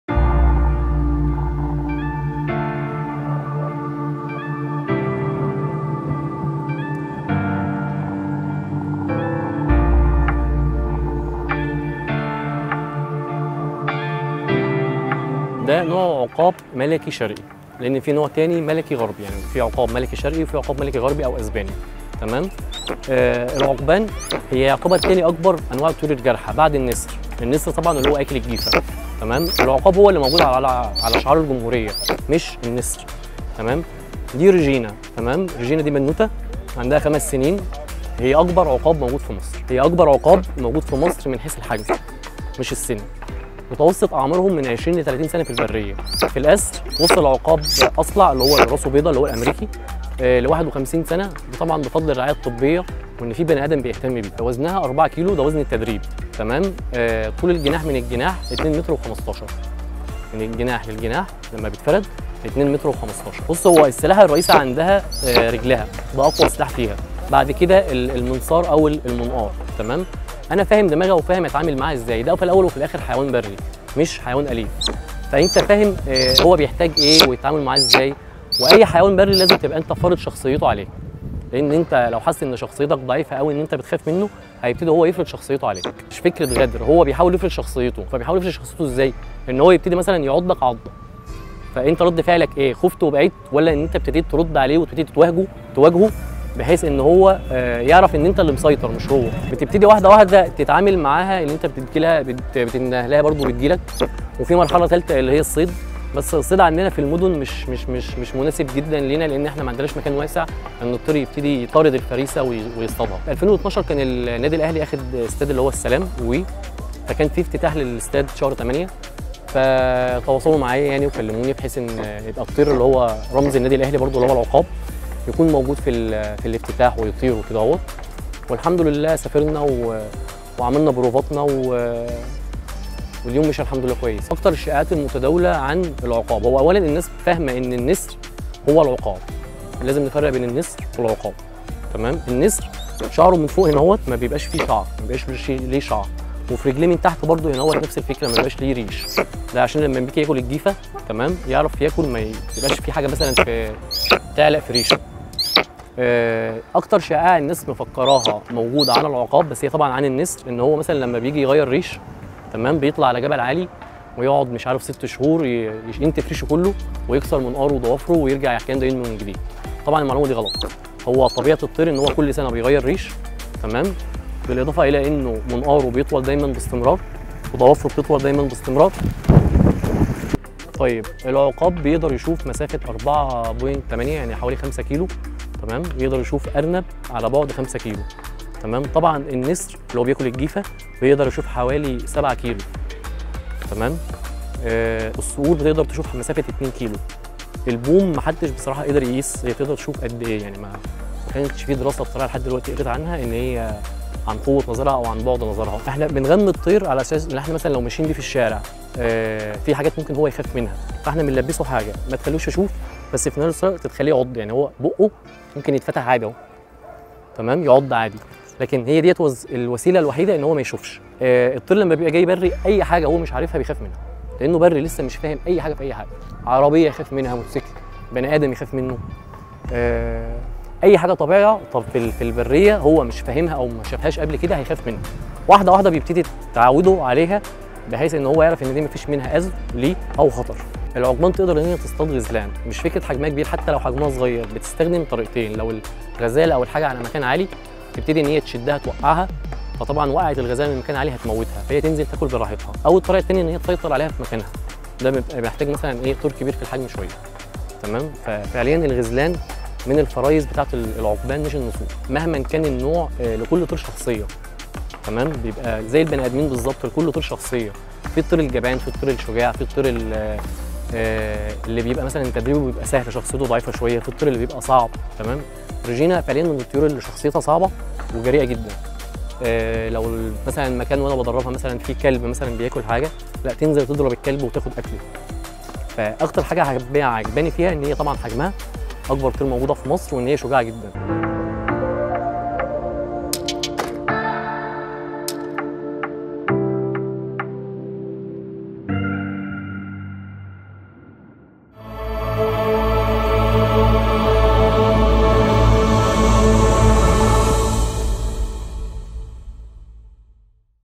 روجينا أكبر عقاب في مصر .. بتصطاد غزلان وبتجوز عن حب ومش بتغدر بصاحبها
ElBaladOfficial#
ElBalad_Official#

ـــــــــــــــــــــــــــــــــــــــــــــــ